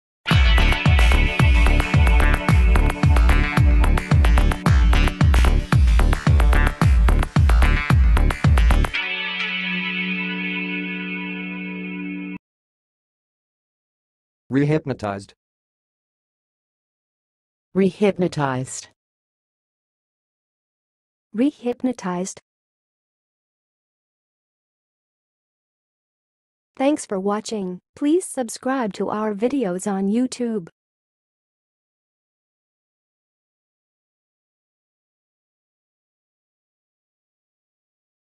HOW TO SAY REHYPNOTIZED? QUICK AND EASY!
REHYPNOTIZED - All About Rehypnotized: How to Say, Definition, and Use It in 2023

Rehypnotized refers to the act of hypnotizing someone again after they have already been hypnotized.
It involves inducing a trance-like state in a person using various techniques and suggestions.
Rehypnotizing is often used in therapeutic settings to reinforce positive changes or to help individuals overcome particular issues or behaviors.
The process is typically conducted by a trained hypnotist or hypnotherapist.

Pronunciation:
To pronounce 'rehypnotized', you can break it down into syllables as re-hyp-no-tized.
Alternatively, you can pronounce it as ree-hyp-nuh-tahyzd.

Test yourself by speaking the following examples:
1. She had such a positive experience with hypnotherapy that she decided to get rehypnotized to reinforce the changes she had made.
2. The hypnotist rehypnotized the patient to help them overcome their fear of flying.
3. After being rehypnotized, he felt a deep sense of relaxation and calmness.
4. Rehypnotizing can be an effective tool for addressing unhealthy habits and behaviors.
5. She was amazed at how easily she could be rehypnotized and enter a trance-like state